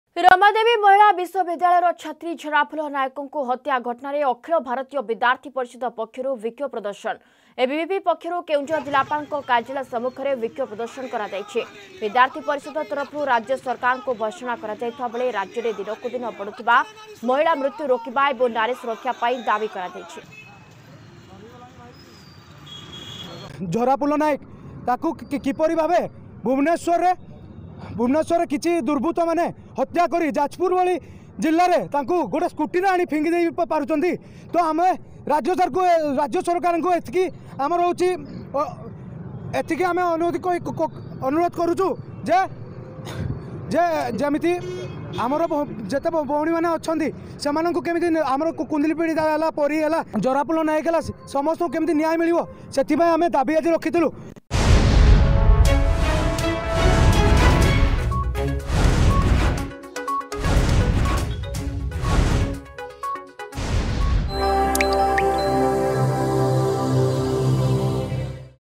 Strike in front of the Kendujhar district commissioner's office by ABVP
ରମାଦେବୀ ମହିଳା ବିଶ୍ଵବିଦ୍ୟାଳୟର ଛାତ୍ରୀ ଝରାଫୁଲ ନାୟକଙ୍କୁ ହତ୍ୟା ଘଟଣାରେ ଅଖିଳ ଭାରତୀୟ ବିଦ୍ୟାର୍ଥୀ ପରିଷଦ ପକ୍ଷରୁ ବିକ୍ଷୋଭ ପ୍ରଦର୍ଶନ । ଏବିଭିପି ପକ୍ଷରୁ କେନ୍ଦୁଝର ଜିଲ୍ଲାପାଳଙ୍କ କାର୍ଯ୍ୟାଳୟ ସମ୍ମୁଖରେ ବିକ୍ଷୋଭ ପ୍ରଦର୍ଶନ କରାଯାଇଛି .